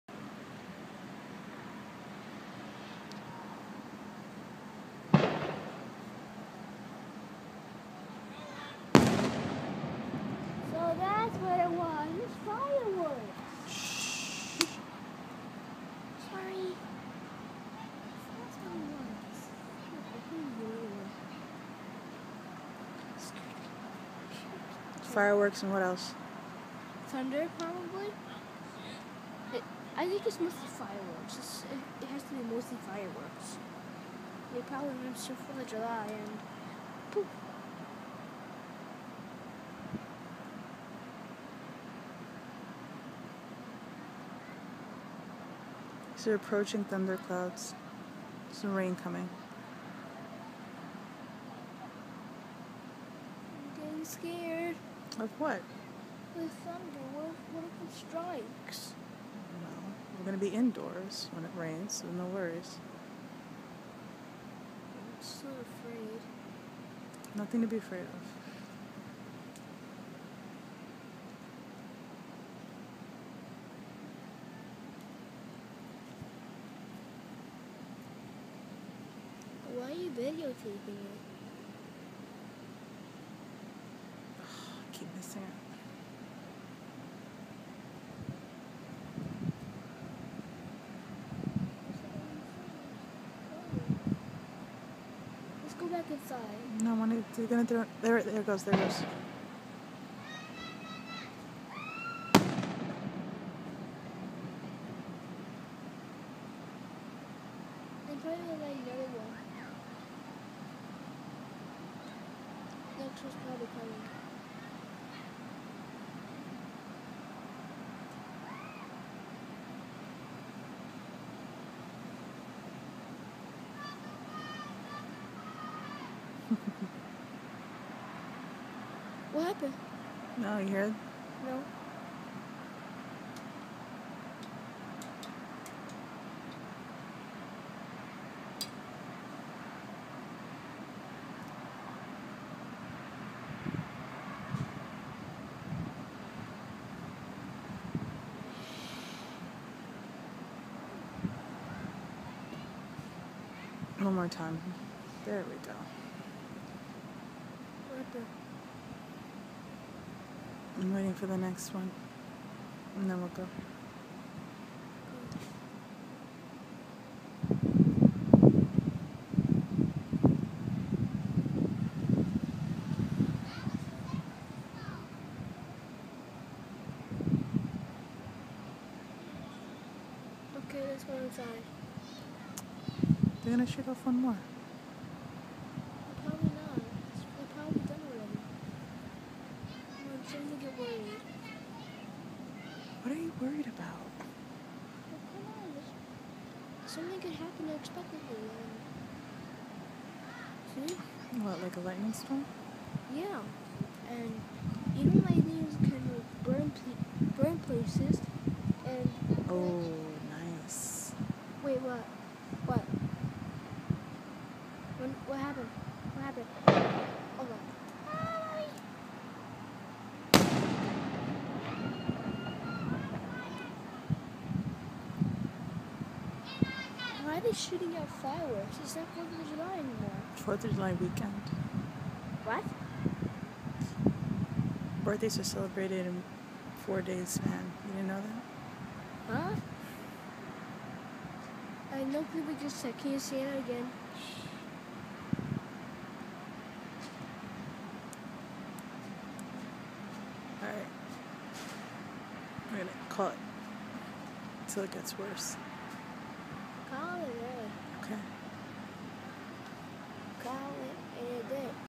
Lightning and Fireworks July 7th
Heard some booms and found out they were fireworks. Then realized there were approaching storm clouds and caught some of nature's fireworks.